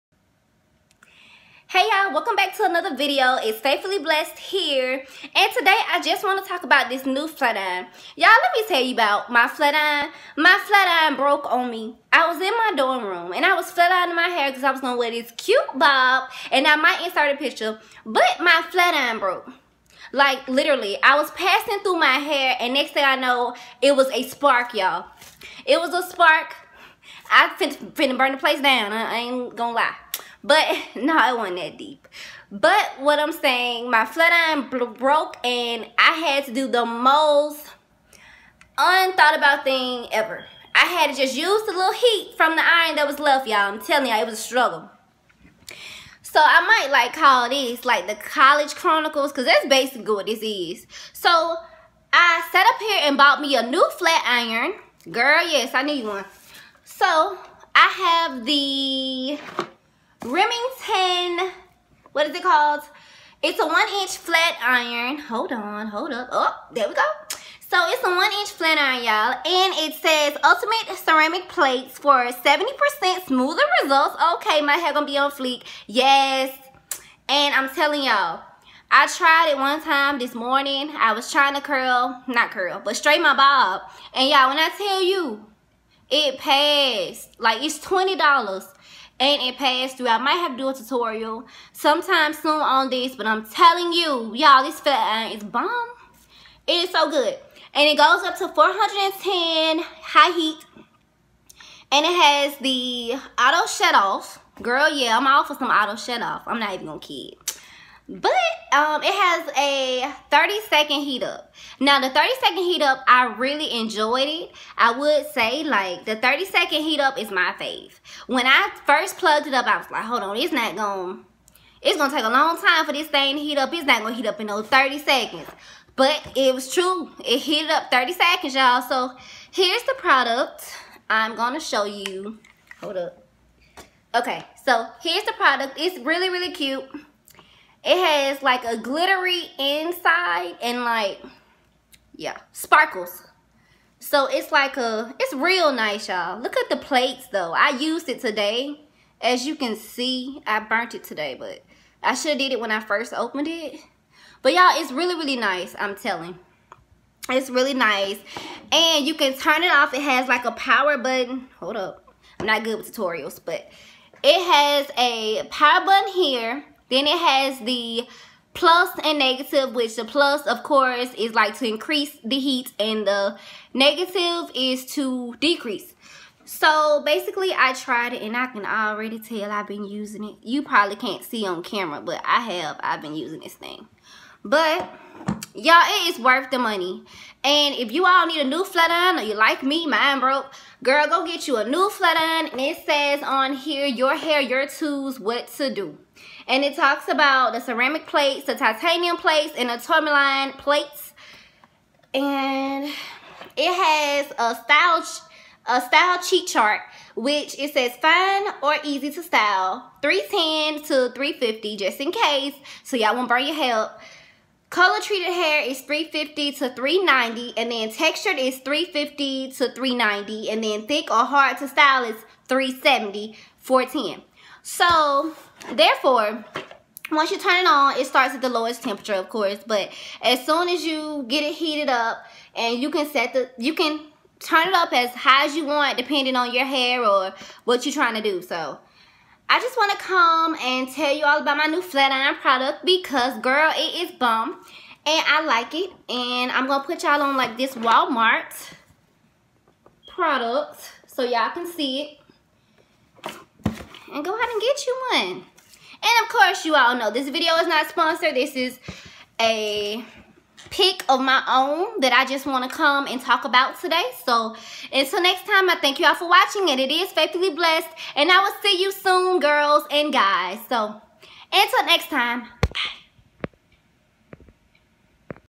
Storytime : Broken flatiron / Remington Flat Iron Review
Hello loves! Tune in to this storytime and this bomb flatiron review!!